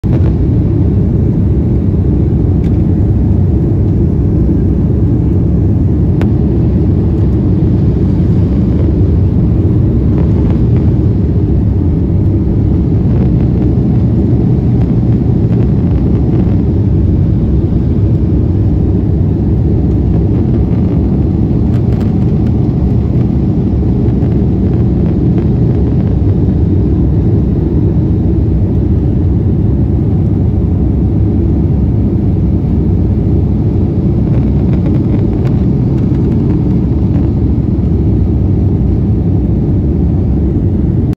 Doha sea view from aircraft
27-Nov-2022 Sunday. Beautiful sea view captured while aircraft took off from Doha to Stockholm.
Courtesy Finnair